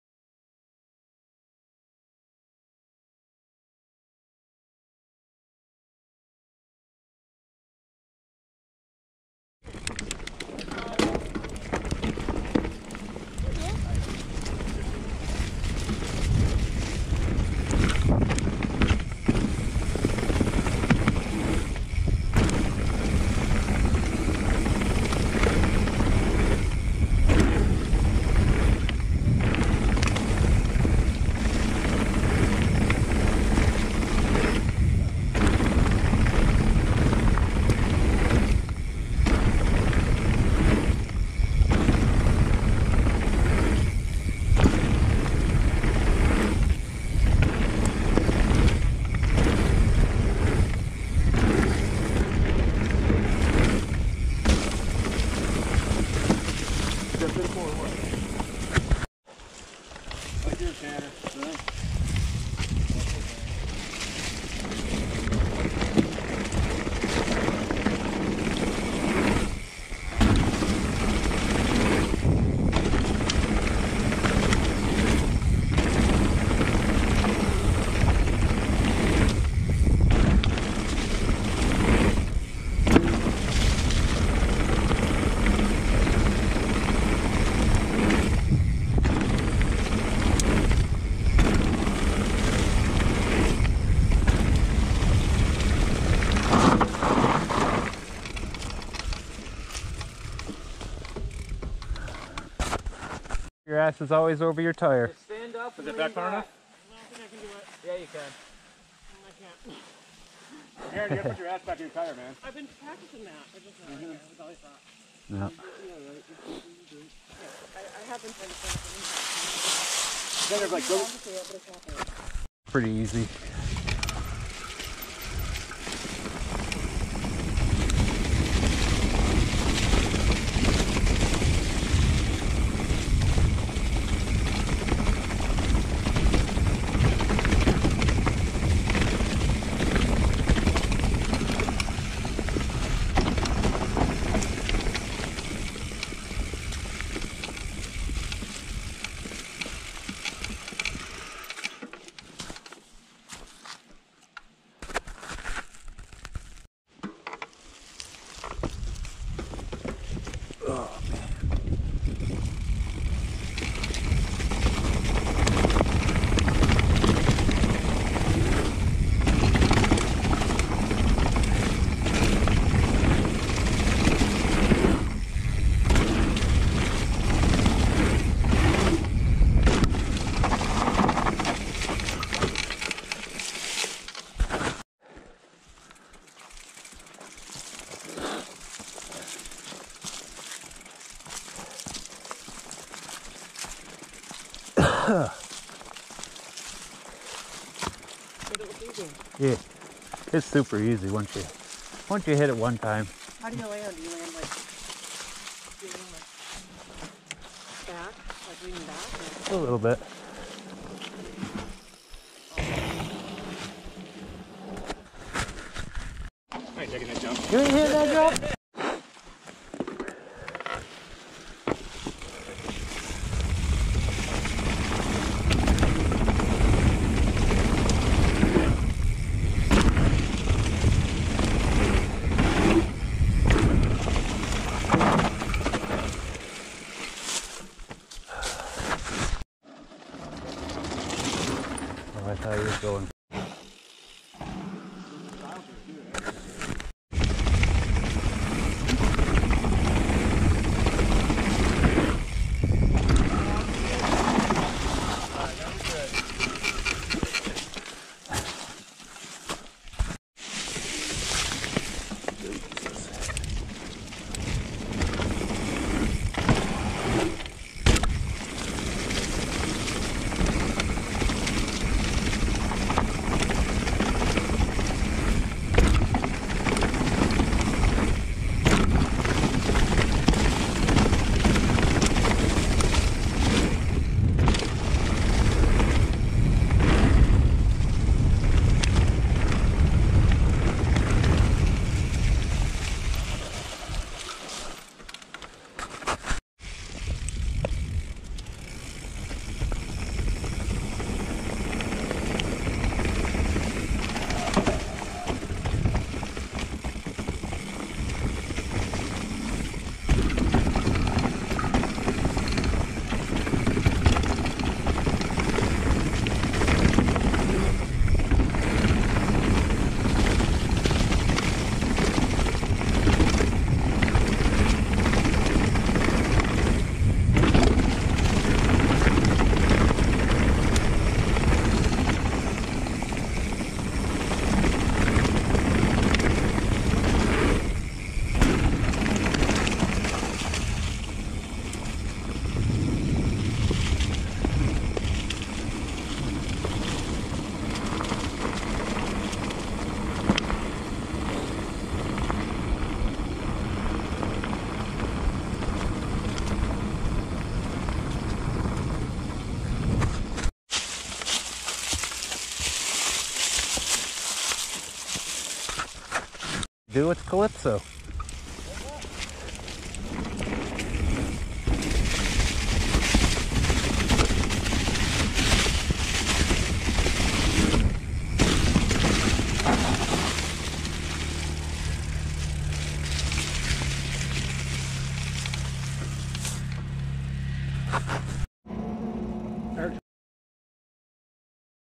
Greek Peak Bike Park - Free Bird
Run down Free Bird, had to hit the boner jump multiple times.